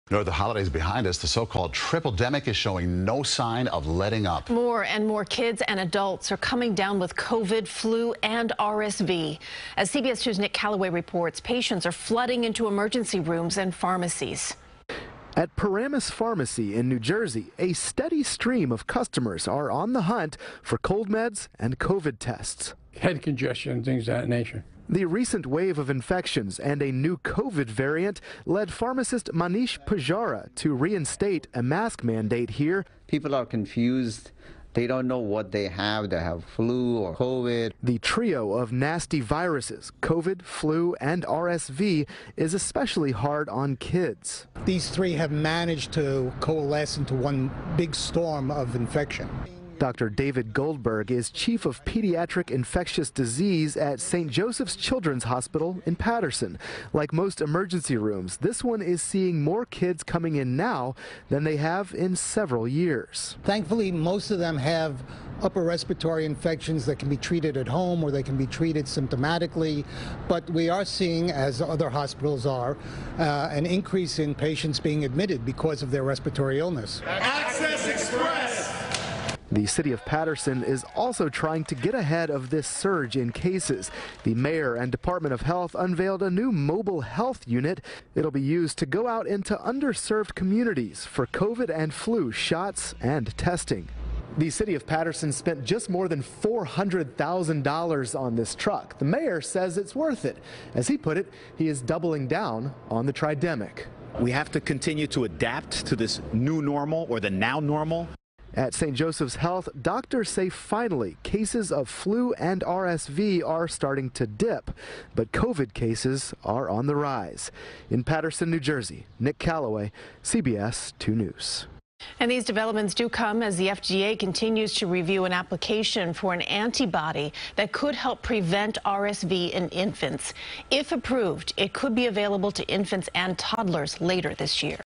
Paterson trying to get out ahead of nasty tripledemic
Doctors say more and more kids and adults are coming down with COVID​, the flu​ and RSV, CBS2's Nick Caloway reports.